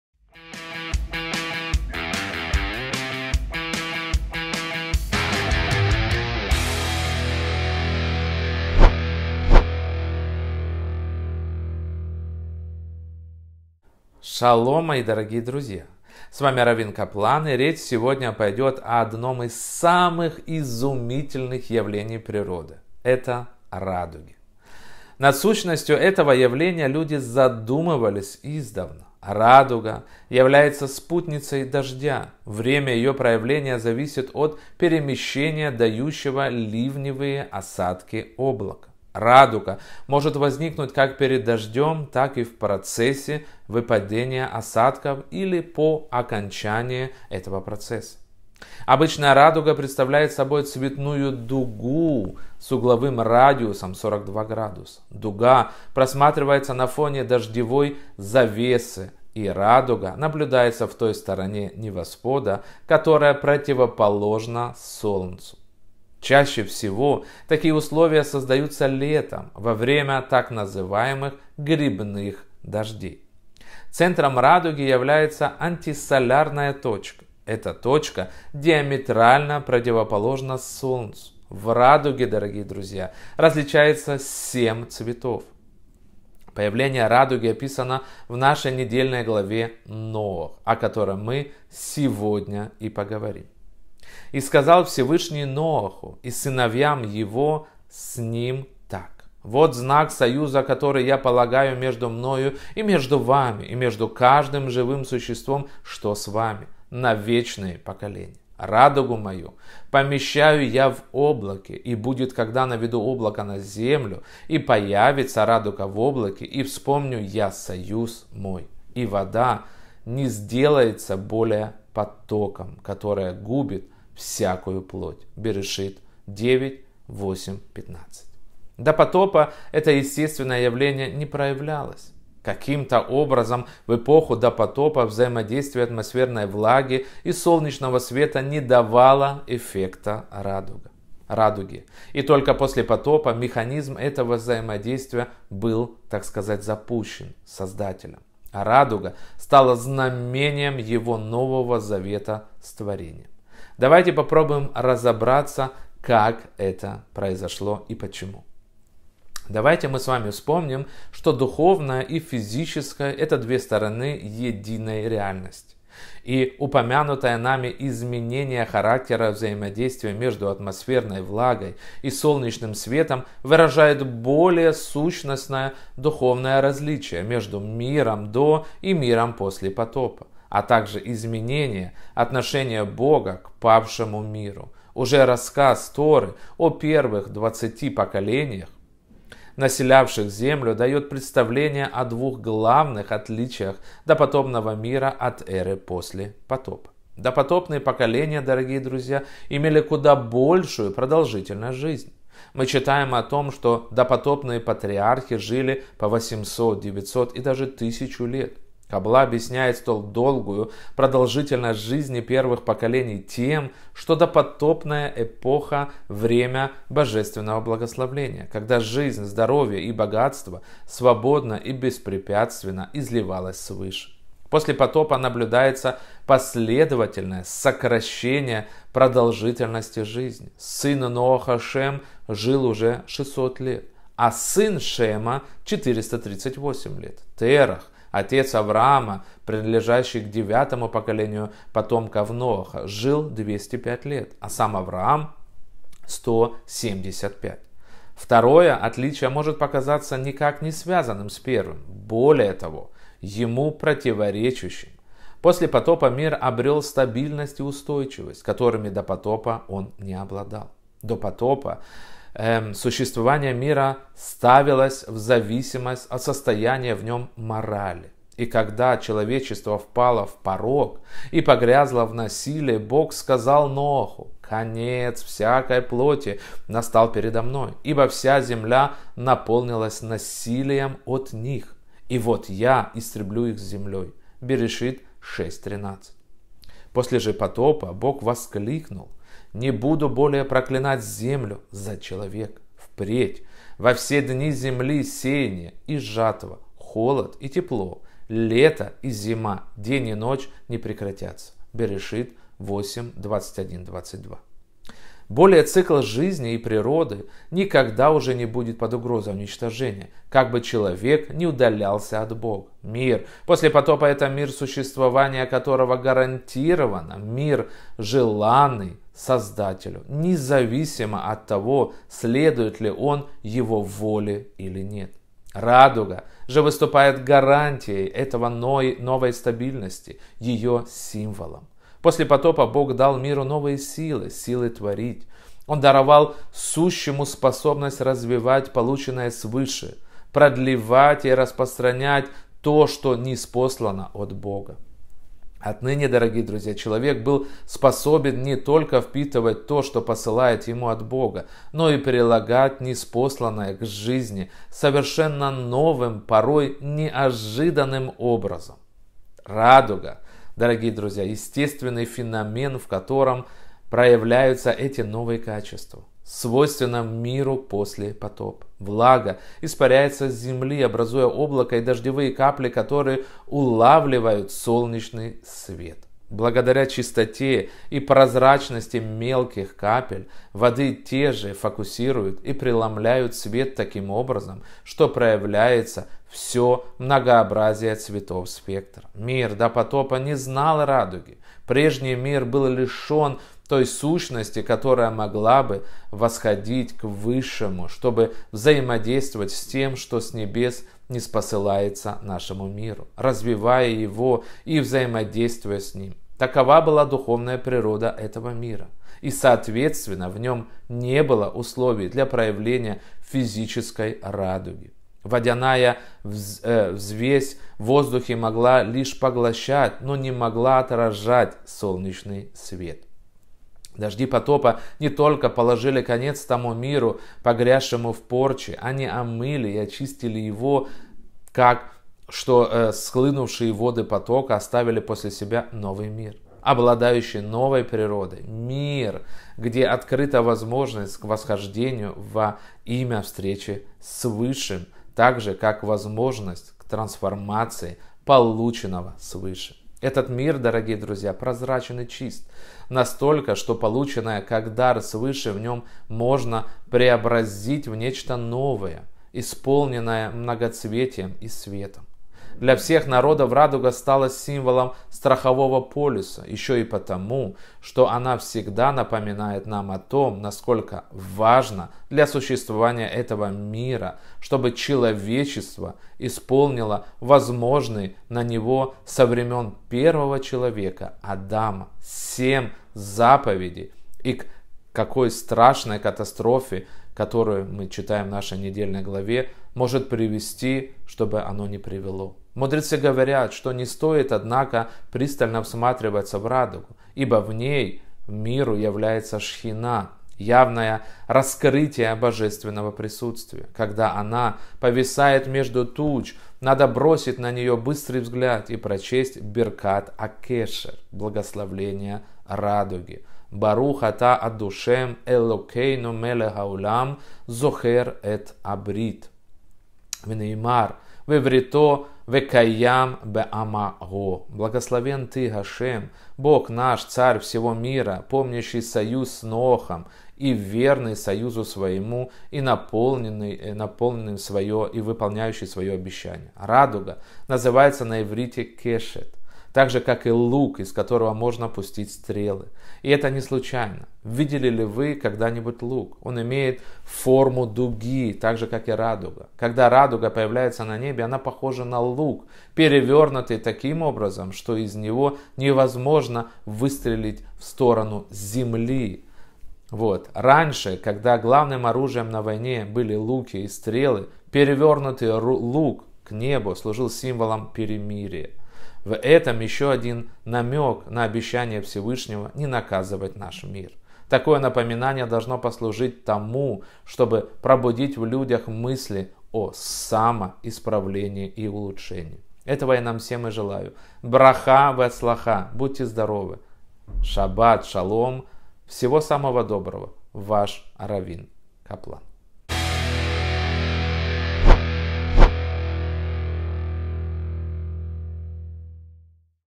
🌈 Радуга - символ перемирий и гарантия жизни на земле. Недельная глава Ноах | Рав Бенцион Каплан
Радуга называется на иврите кешет – так же, как лук, из которого можно пускать стрелы. И это не случайно! Видели ли вы когда-нибудь лук? Он имеет форму дуги – так же, как радуга. Когда радуга появляется на небе, она похожа на лук, перевернутый таким образом, что из него невозможно “выстрелить” в сторону земли – раньше, когда главным оружием на войне были луки и стрелы, перевернутый лук служил символом перемирия! В этом еще один намёк на обещание аШема не наказывать мир.

Такое напоминание должно послужить тому, чтобы пробудить в людях мысли о самоисправлении. Этого я нам всем и желаю! Браха вехацлоха! Будьте здоровы! Шаббат шалом!
Ваш раввин Каплан